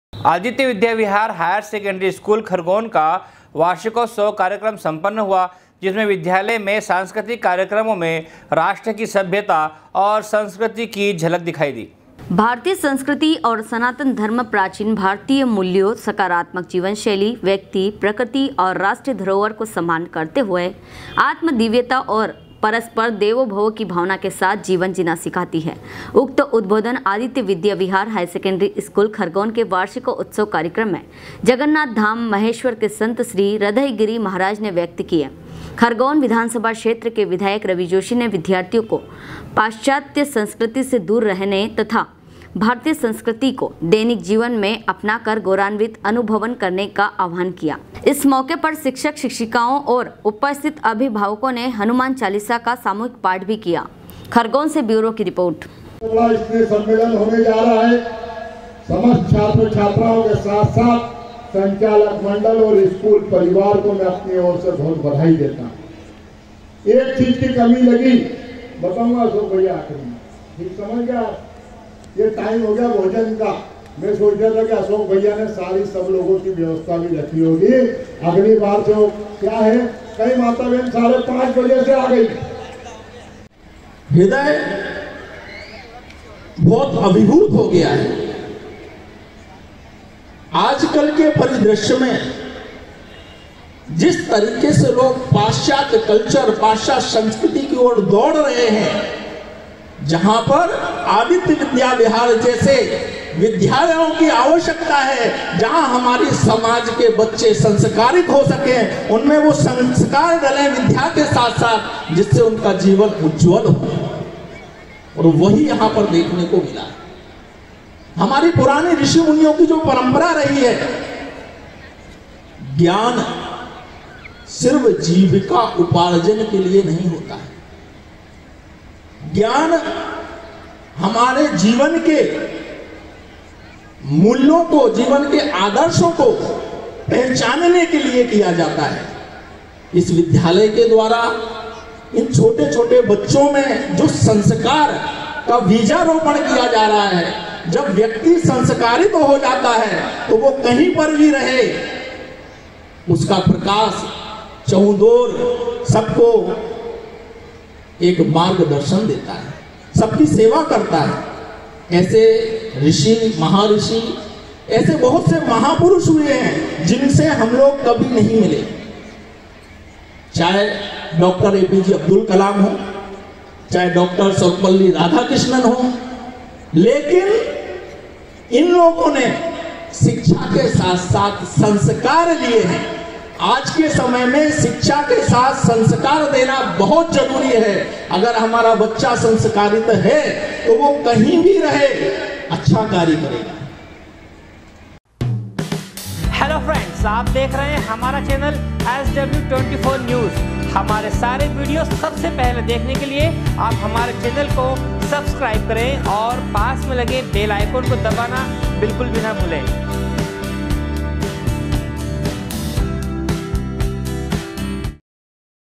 आदित्य विद्या विहार हा.से. स्कूल खरगोन का वार्षिकोत्सव कार्यक्रम संम्पन्न हुआ
आदित्य विद्या विहार हा.से. स्कूल खरगोन का वार्षिकोत्सव कार्यक्रम संम्पन्न हुआ जिसमें विद्यालय में सांस्कृतिक कार्यक्रमो में राष्ट्र की सभ्यता और संस्कृति की झलक दिखाई दी।